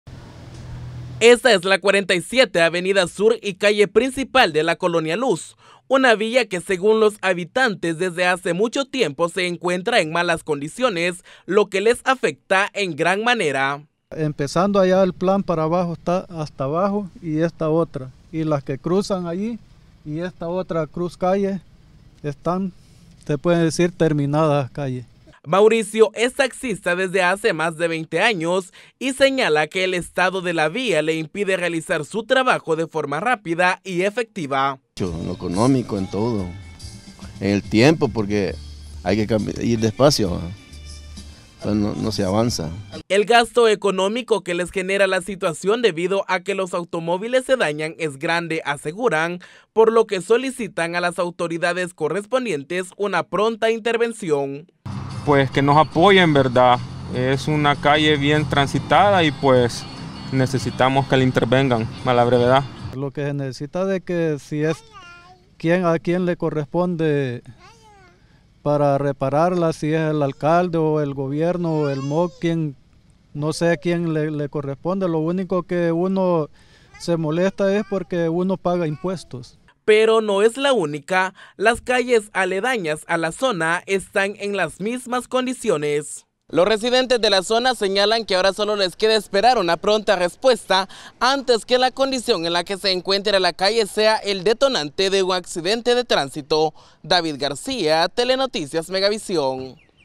CALLE EN MAL ESTADO
Las calles en mal estado son un problema al que día a día se enfrentan muchos conductores en diferentes puntos del país, tal es el caso de la calle principal de la colonia Luz.